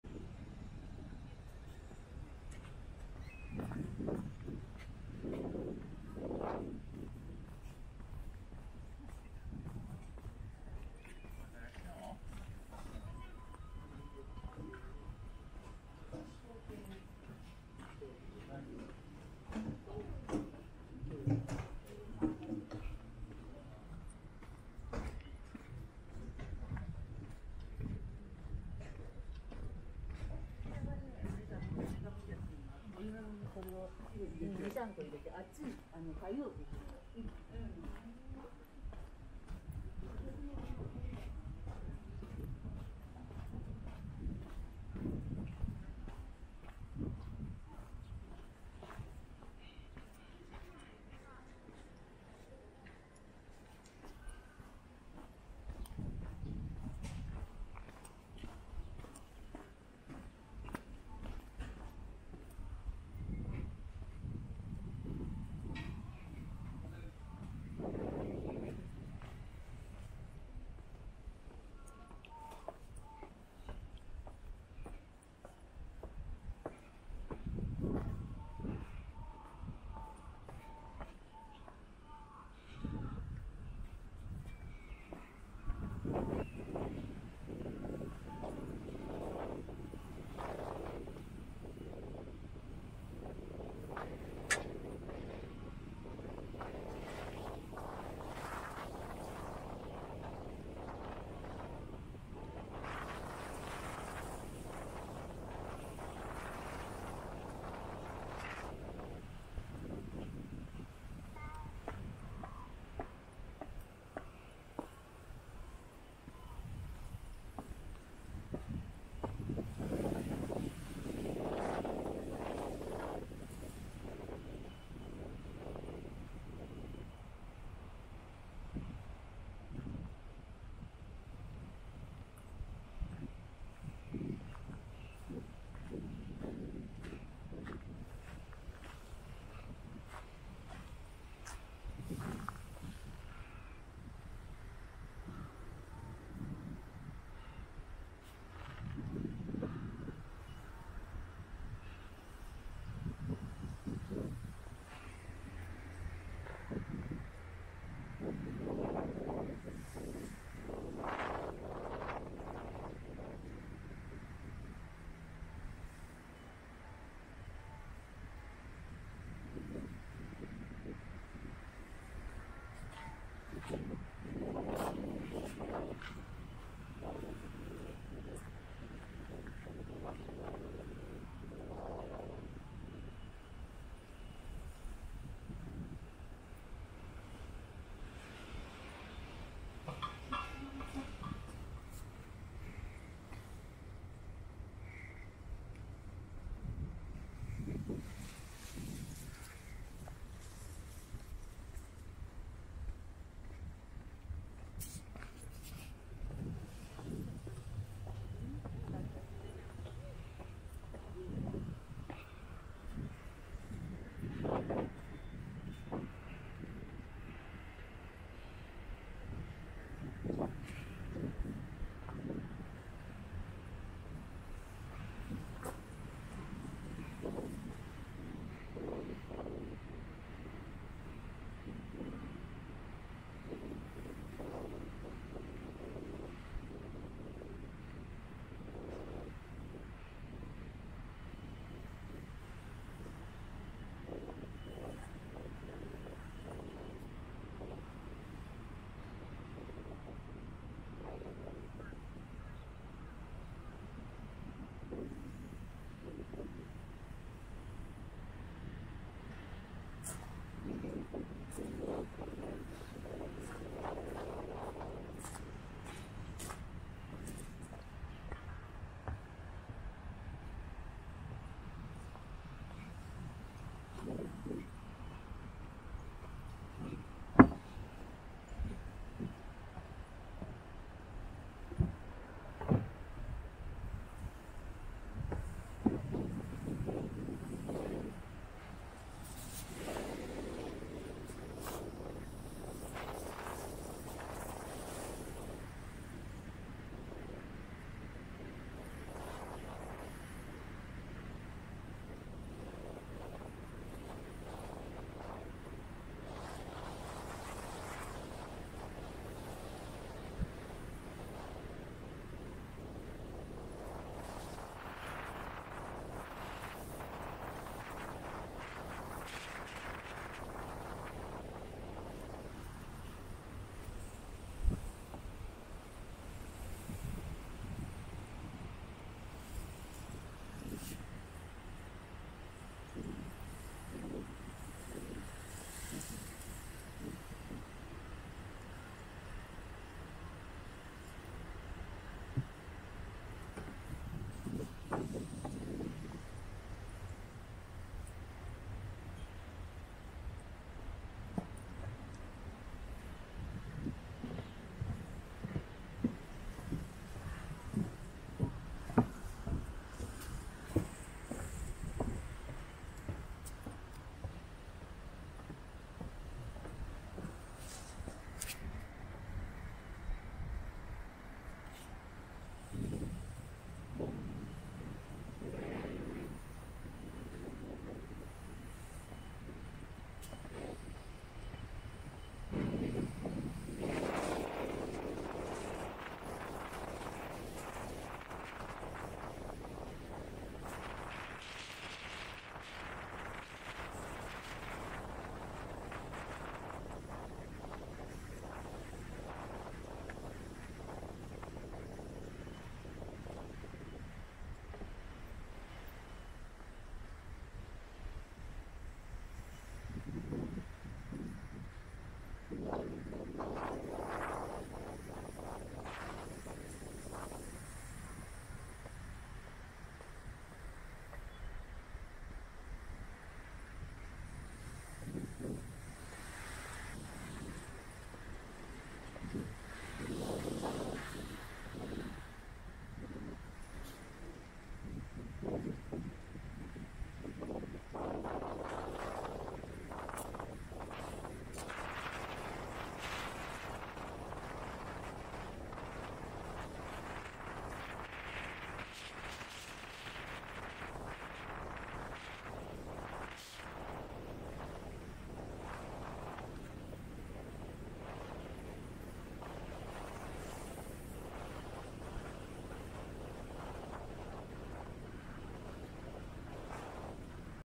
Walk in Jogashima Lighthouse Park in Miura City, Kanagawa Prefecture
Connected by the Jogashima Ohashi Bridge from the southern tip of the Miura Peninsula, Jogashima is a small isolated island that can be reached in an hour by car from Yokohama.
The famous spot of Jogashima originated in the middle of the 1600s, and it is the Jogashima lighthouse located in the west of the island which was rebuilt in 1925.
This time, we took a walk from the Jogashima Lighthouse Shopping Street to the Jogashima Lighthouse Park.

Please note that the sound of the wind is strong due to the nature of the place.

*EQUIPMENT*
▶SD card